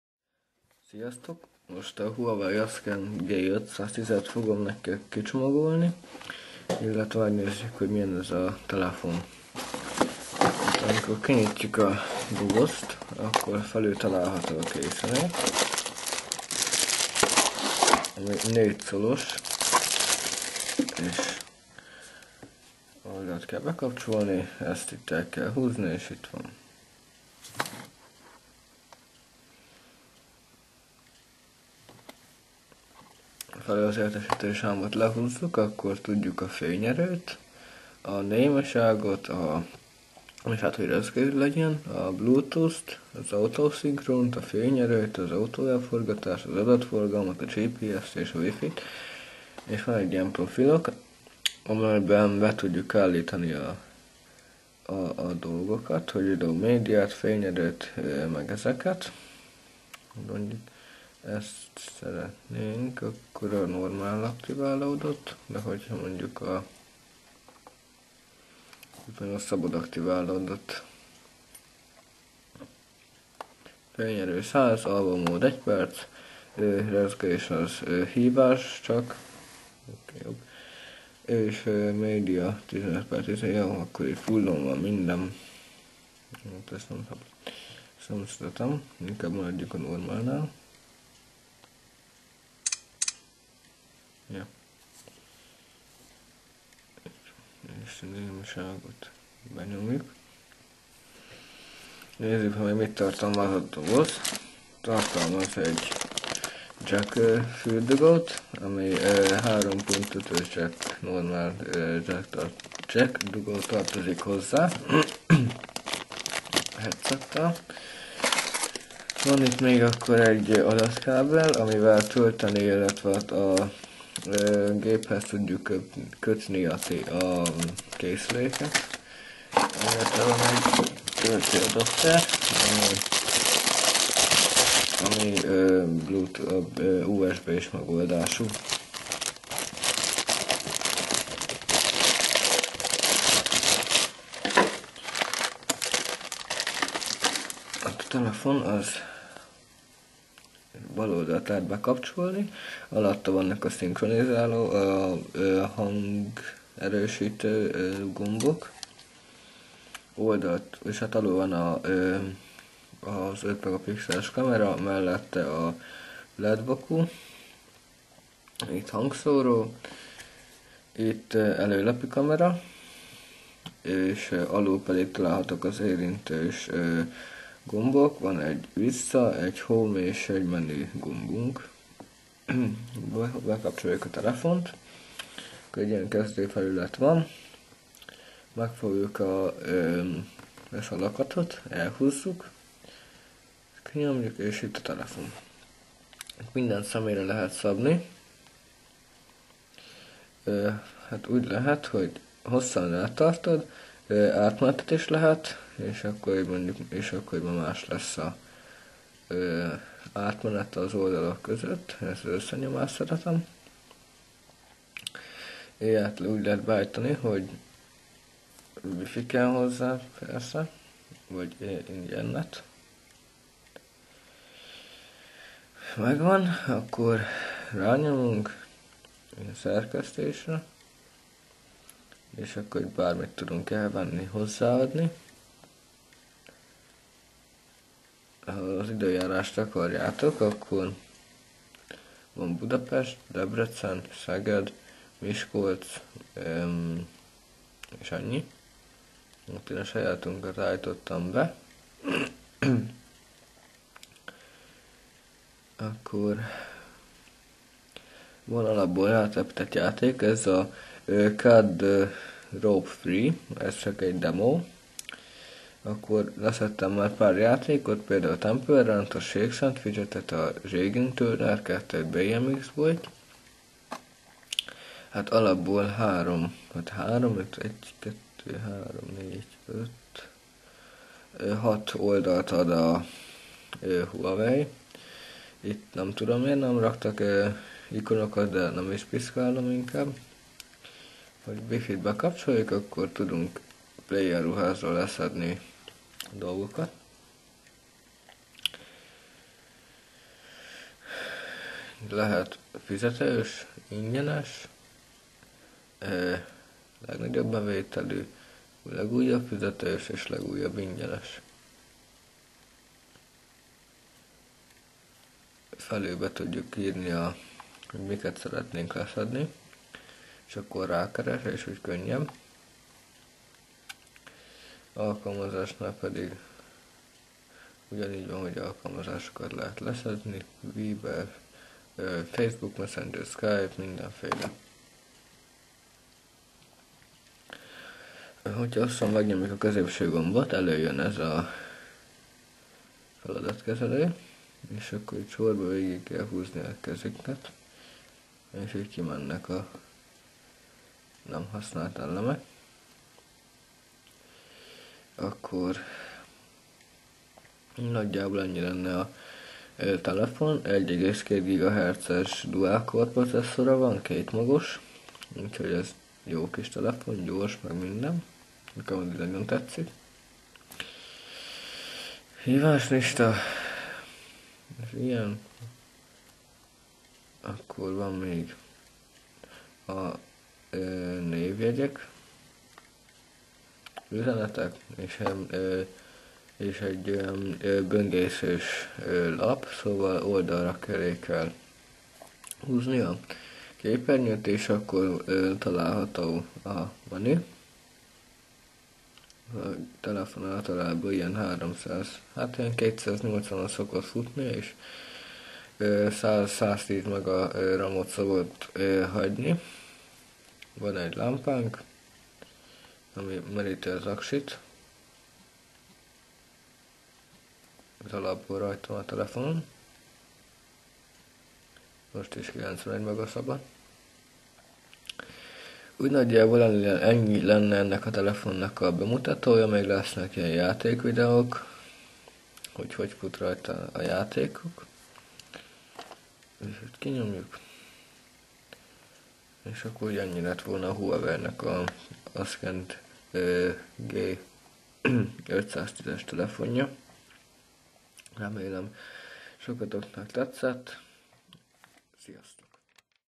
Huawei Ascend G510 Unboxing és Bemutató (HUN) (HD)
Rövid kicsomagolás és bemutató videója a Huawei Ascend G510-es telefennak.
Ha kérdésed van, írj kommentet!:)